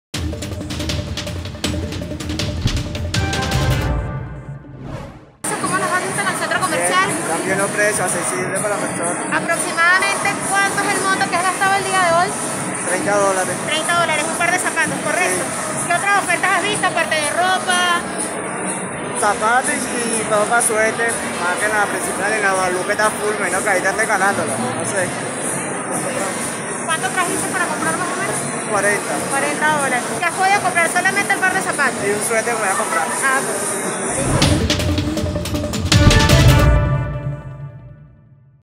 Periodista Mariana Yépez reporta cómo se vive el Black Friday en Caracas
El Black Friday o «viernes negro» se instaló definitivamente en Venezuela. Se trata de una costumbre consumista estadounidense, que ha sido exportada a casi el mundo entero. Comenzó a aplicarse en Filadelfia alrededor de 1961 y luego se expandió por Europa y en los últimos años por América.